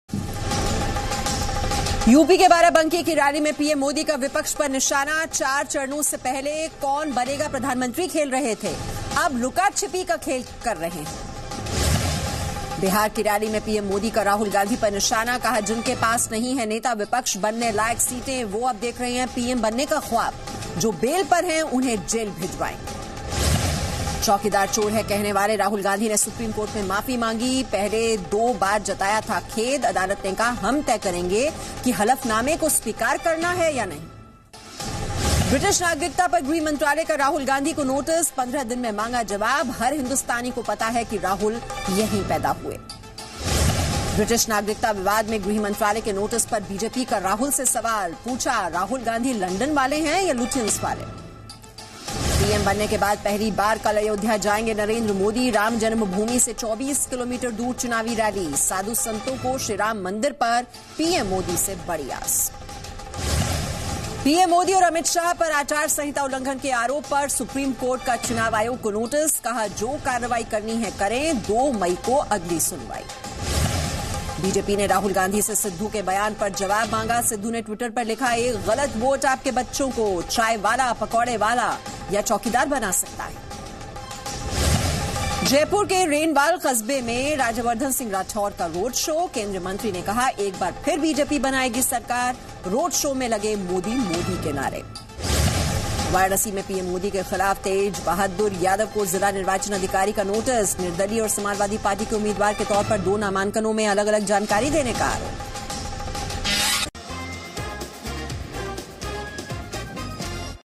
Watch top 10 news stories related to Lok Sabha elections 2019
In this segment of Zee News watch top 10 news related to Lok Sabha elections 2019. Watch this video to know more.